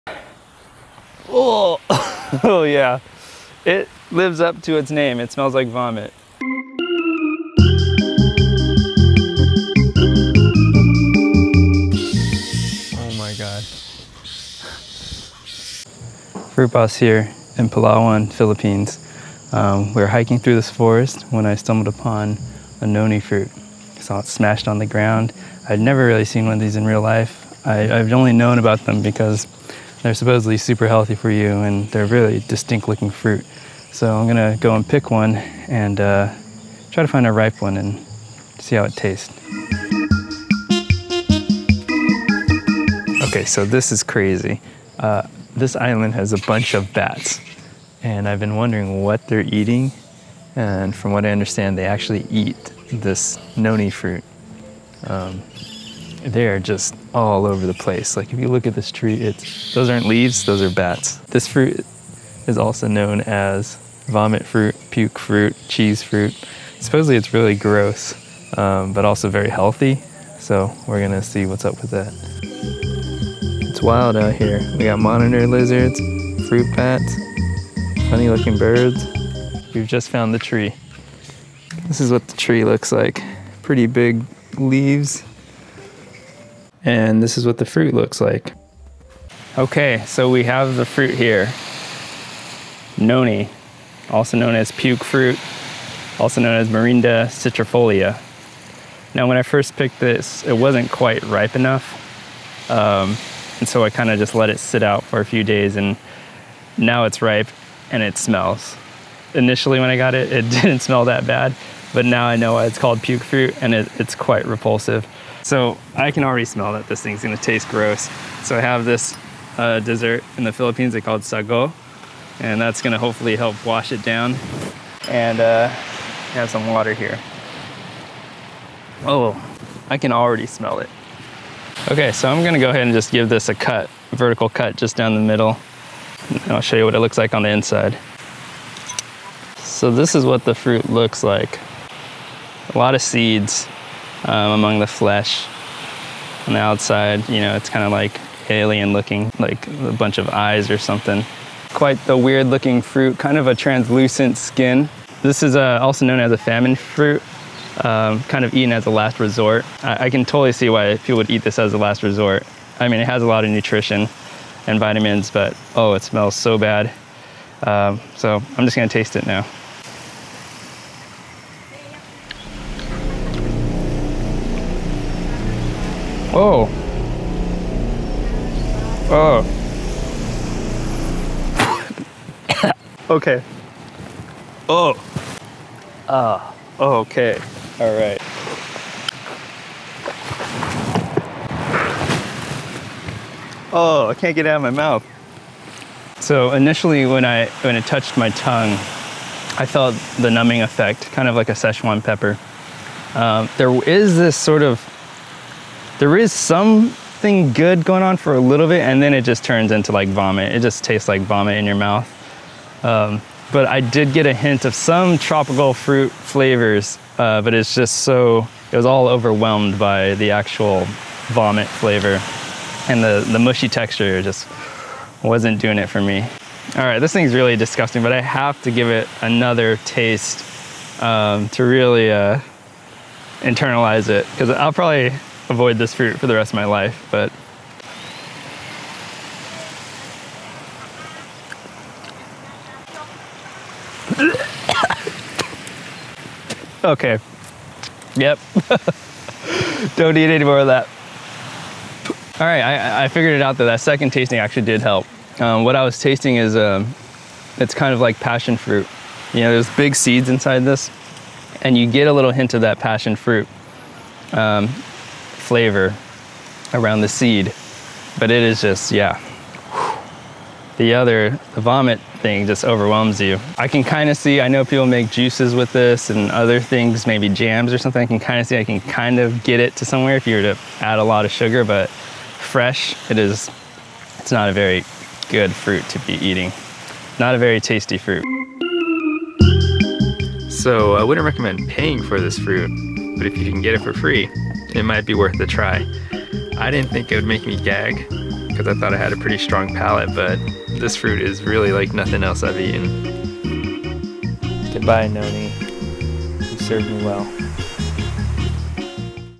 I Will Never Eat This Fruit Again! - Puke Fruit Review (Noni, Vomit Fruit, Morinda Citrifolia)
I try the infamous Puke Fruit and it lives up to its name. Also, known as Noni, this fruit grows readily in the pacific islands. I was fortunate, or rather, unfortunate enough to find a tree during my trip to the Palawan, Philippines.